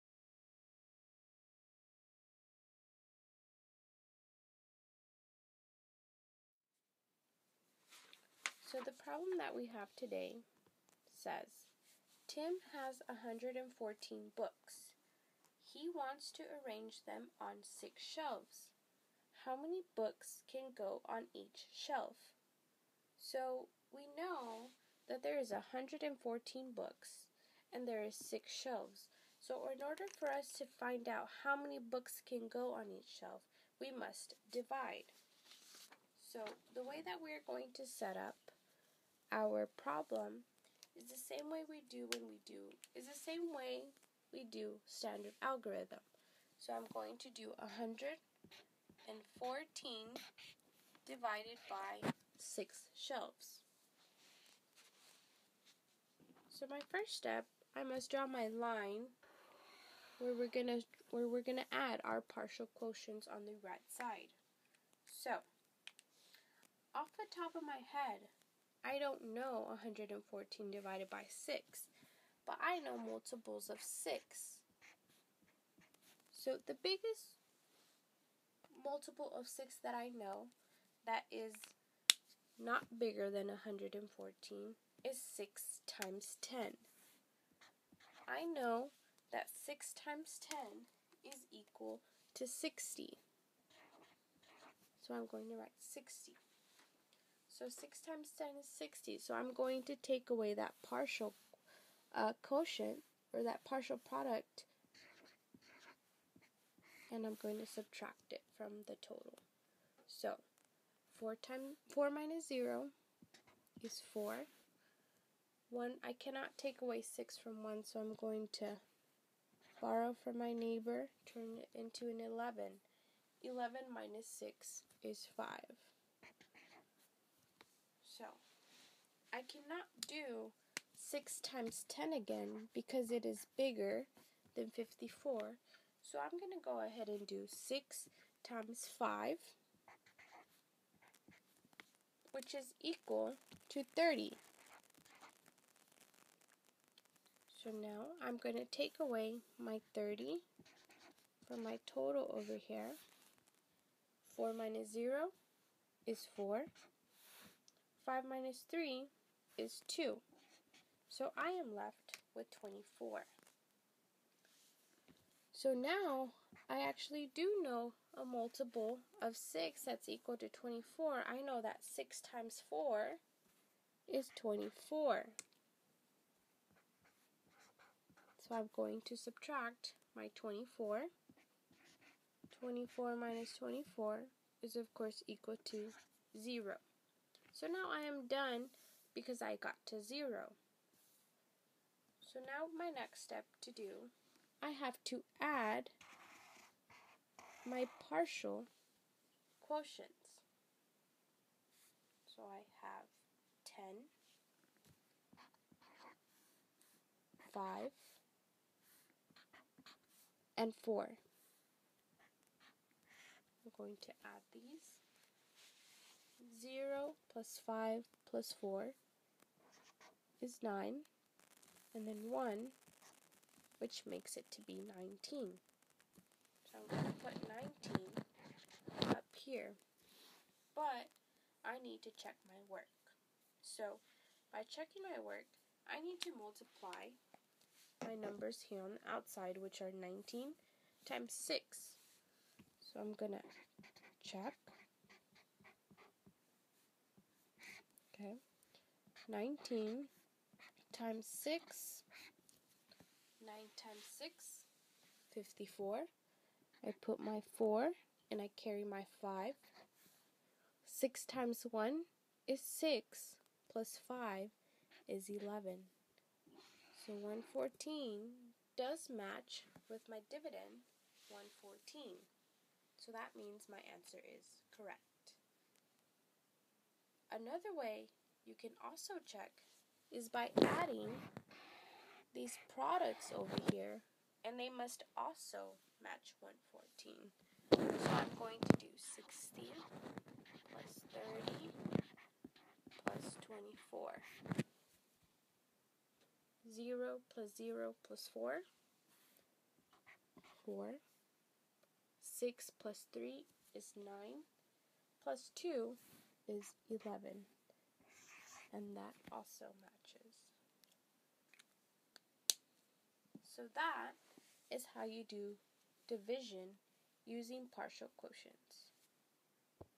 Division: Using Partial Quotients 4th grade
4th grade teacher teaching division using partial quotients.